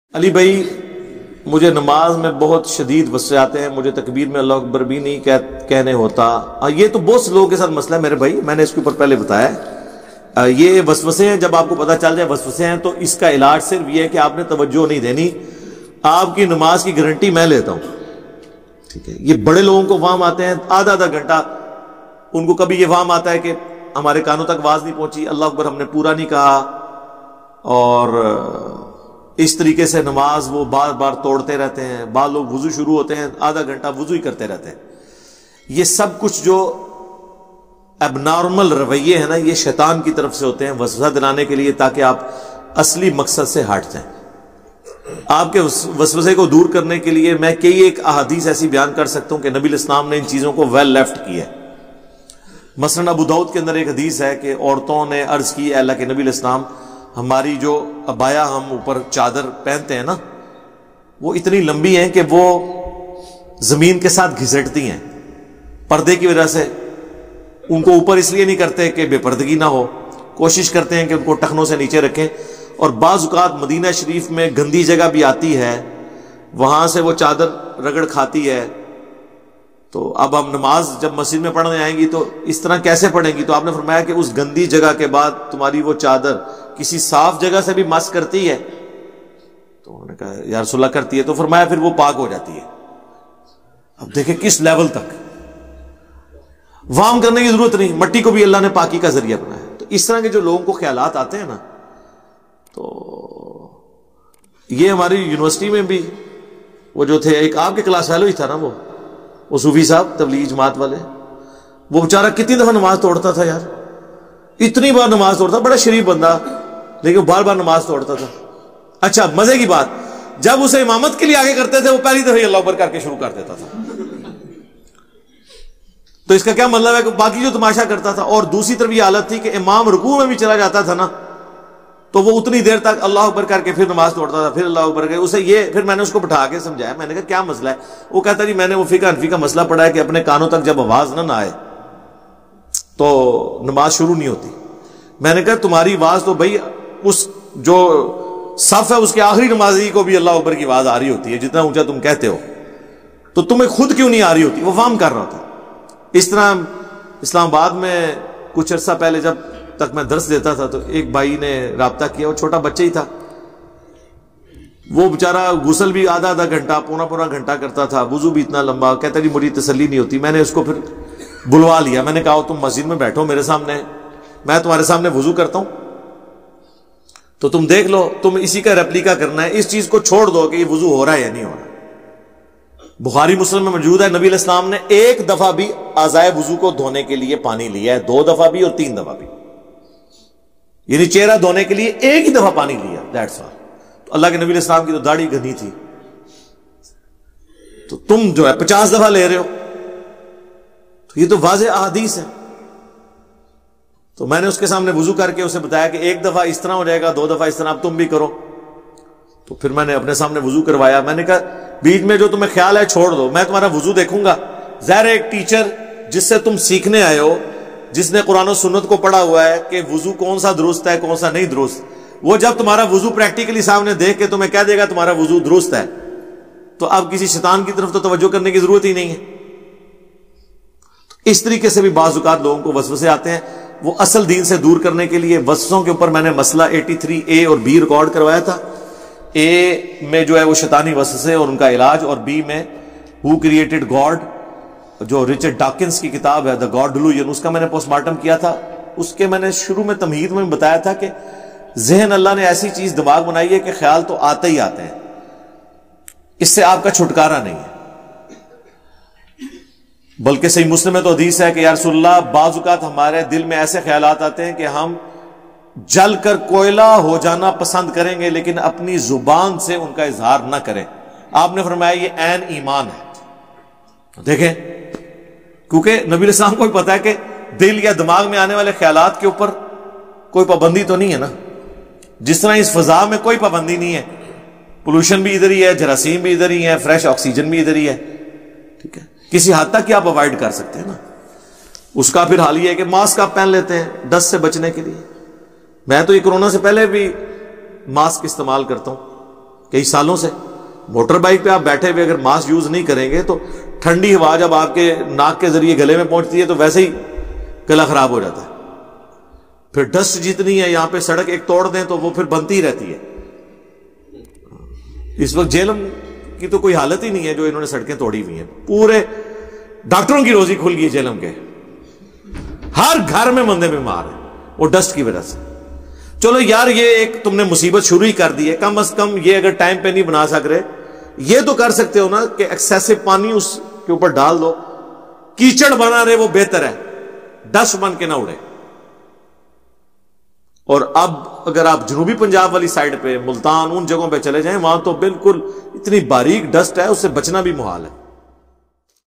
Shaitani Waswaso Ka ilaj Namaz Guarantee Se Qabool Hogi !!! Kitabi Deen[Engineer Muhammad Ali Mirza]
namaz main waswaso ka ana aur is ka ilaj

This channel is all about Islamic education. Jazak Allah